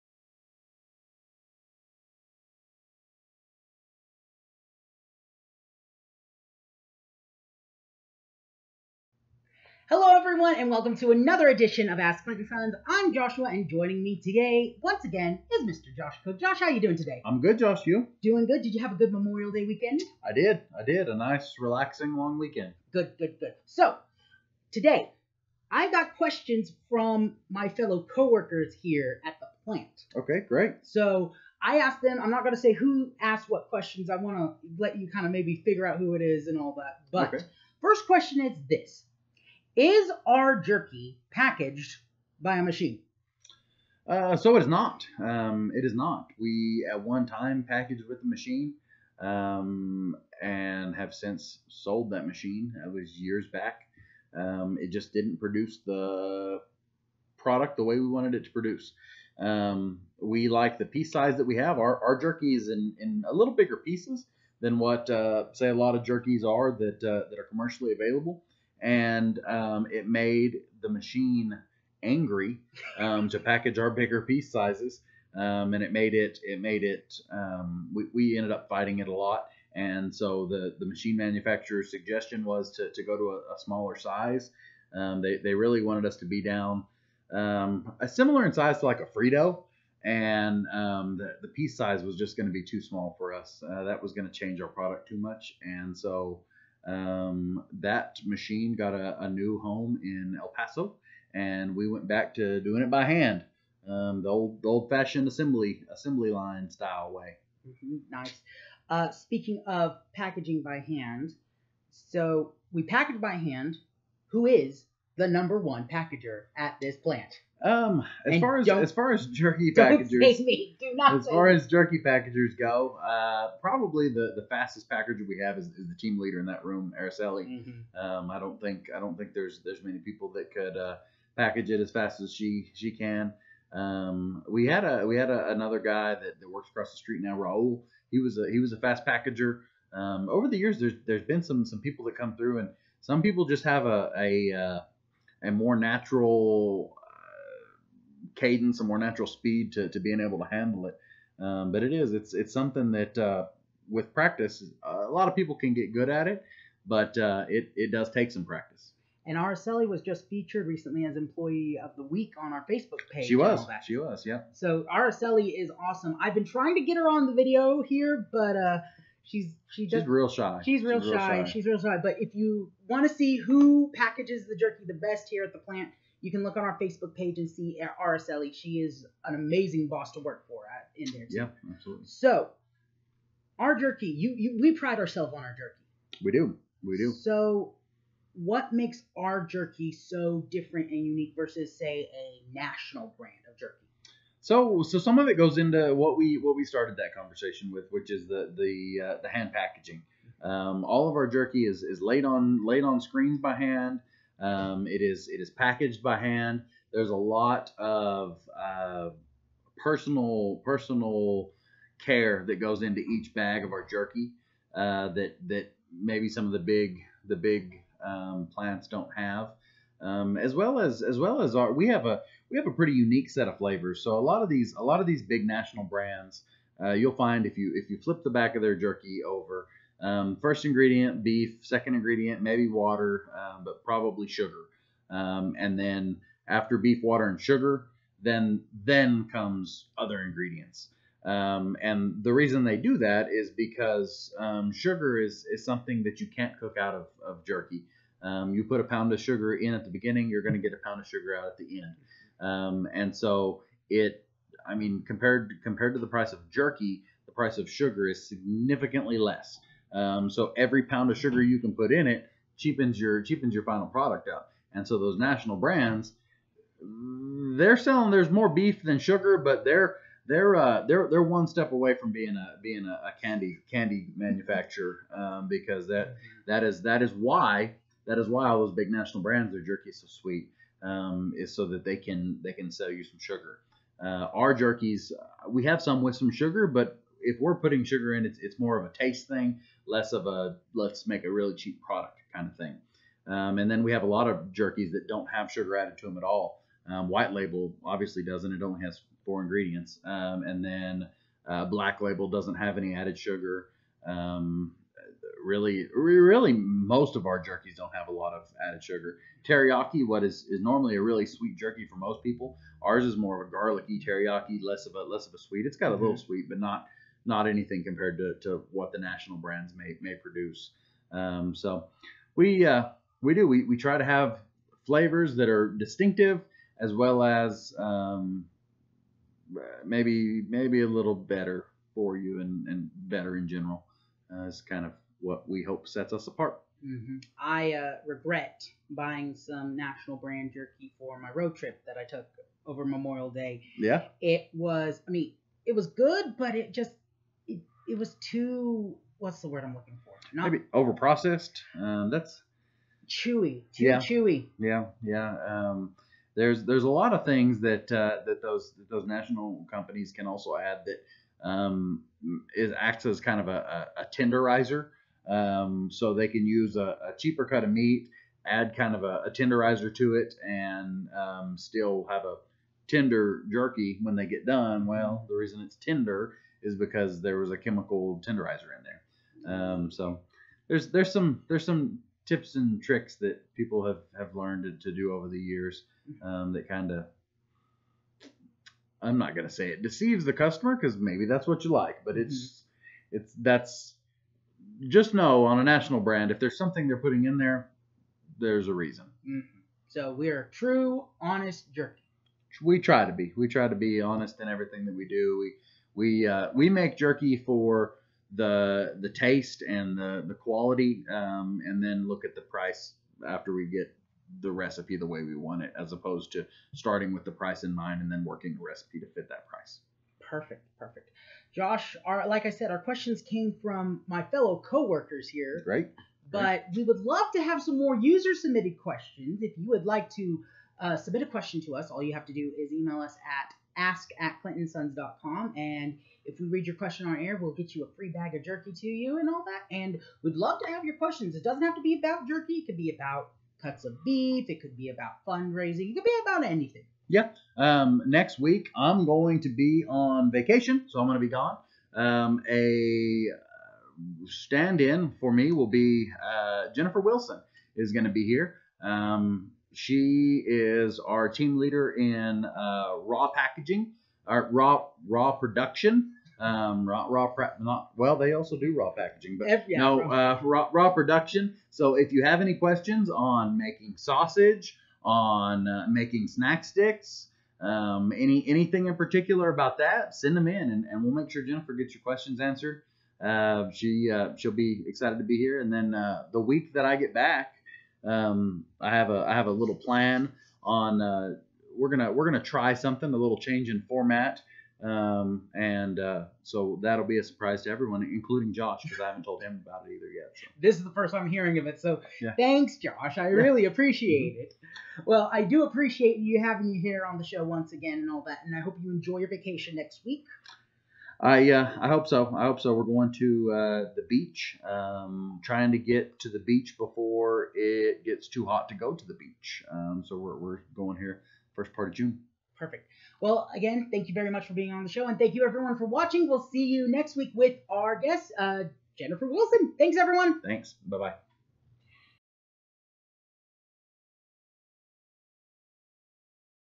Ask Clint & Sons [Episode Fourteen] - Coworkers Ask Questions
It's Ask Clint & Sons!
This is our fourteenth episode of "Ask Clint & Sons!" where WE answer YOUR questions!
This week fellow coworkers ask questions and it's all about jerky!

Try our featured product!